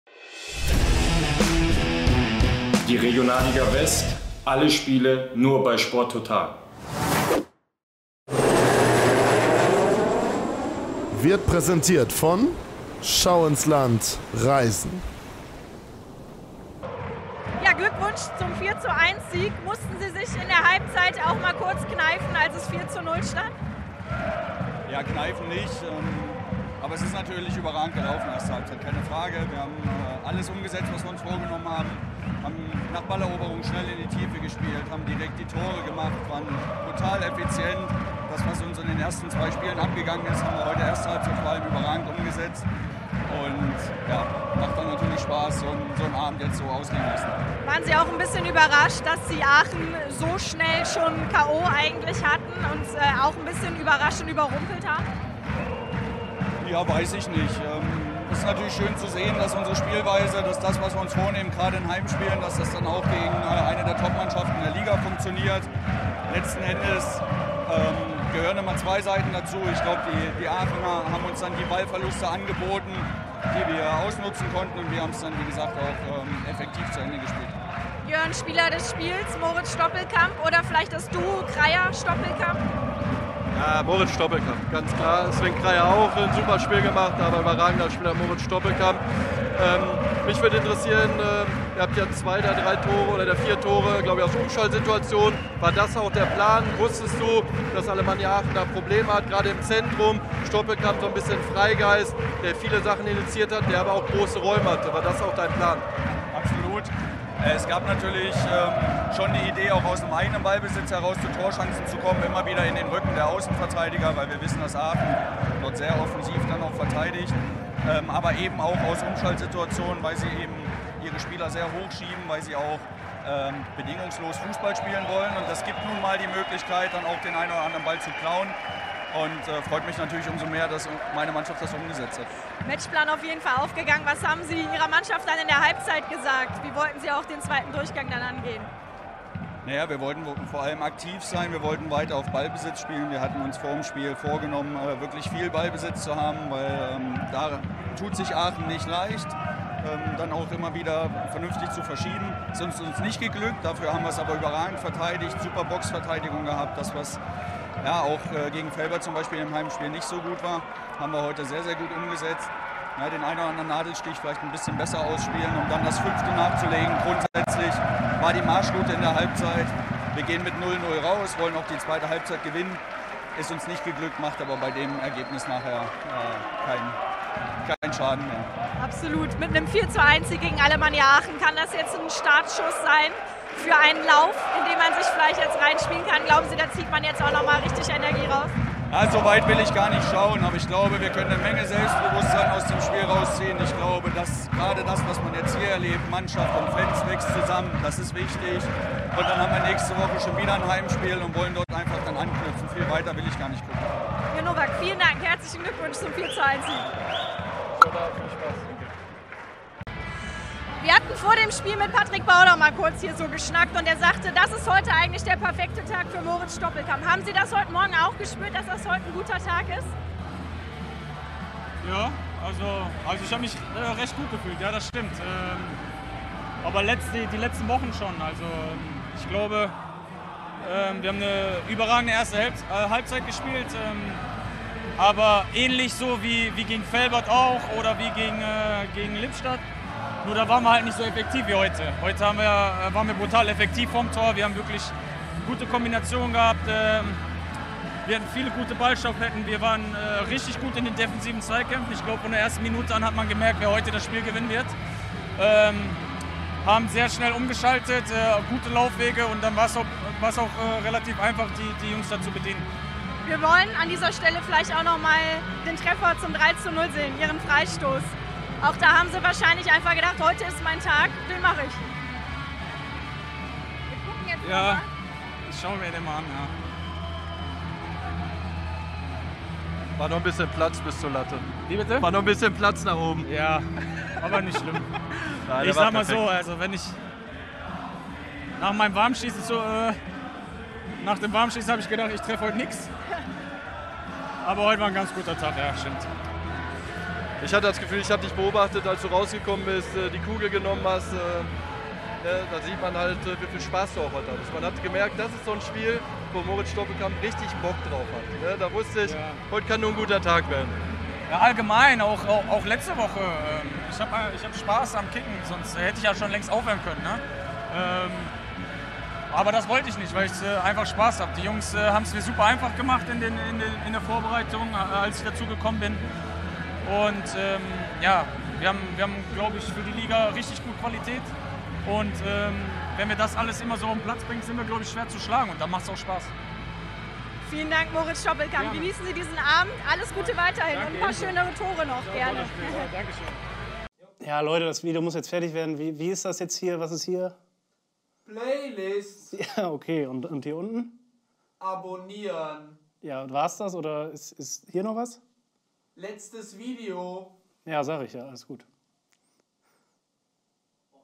"Wir waren brutal effizient!" | Rot-Weiß Oberhausen - TSV Alemannia Aachen | Stimmen zum Spiel
Rot-Weiß Oberhausen - TSV Alemannia Aachen, 4. Spieltag Regionalliga West 2023/24

Am Freitagabend geht es für die Alemannia Aachen nach Oberhausen, wo beide Titelanwärter unter Flutlicht gegen einen bisher schwachen Saisonstart spielen. Sowohl die Aachener als auch die Oberhausener liegen deutlich hinter den Erwartungen, nachdem man beide im Vorfeld zu den Favoriten auf den Aufstieg gezählt hat. Diese Partie könnte auch ein Spiel gegen die erste Trainerentlassung sein. Die sportliche Führung beider Seiten mit den bisherigen Leistungen nicht glücklich sein und schon bald handeln. Für wen wird die Luft dünner?

Tore:
1:0 Kreyer (RWO)
2:0 Kleinsorge (RWO)
3:0 Stoppelkamp (RWO)
4:0 Kreyer (RWO)
4:1 Brasnic (AAC)

Aufstellungen:

Rot-Weiß Oberhausen:
Benz – Fassnacht, Stappmann, Öztürk, Montag (60. Donkor)– Holthaus (32. Yalcin), Ngyombo – Kleinsorge, Stoppelkamp (85. Ruzgis), Kefkir (85. Rexha) – Kreyer
Trainer: Jörn Nowak

TSV Alemannia Aachen:
Johnen – Winter, Uzelac, Rumpf (27. Dervisevic), Strujic – Bapoh (46. Baum), B. Müller (62. Afamefuna), Herzog – Schaub (32. Peters), Brasnic, Heinz (62. Ametov)
Trainer: Helge Hohl

Schiedsrichter: Leonidas Exuzidis
Zuschauer: 5.763

Kommentator: Sven Lesser
Experte: Björn Mehnert